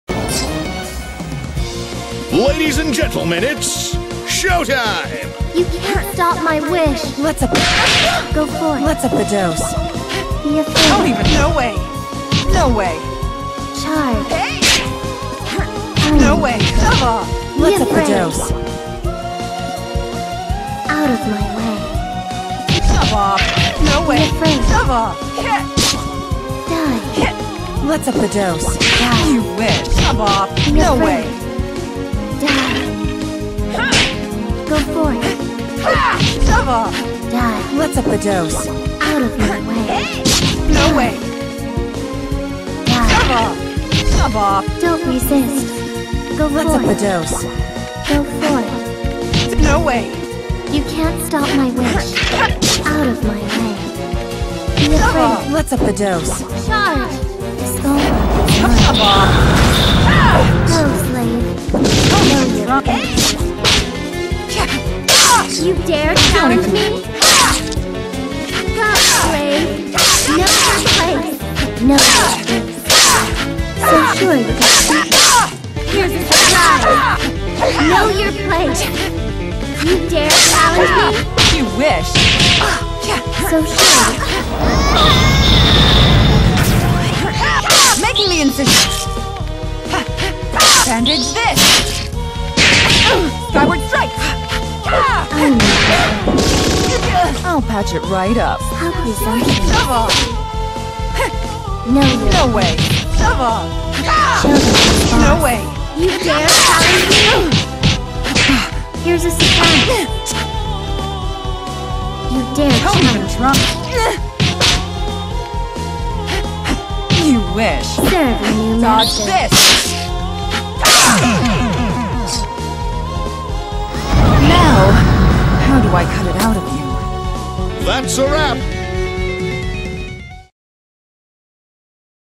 beating marie
"Nightmare" difficulty. Would have been much easier if I hadn't thrown the first two air scalpels from a position where most of them missed. Air scalpels really destroy marie's final form.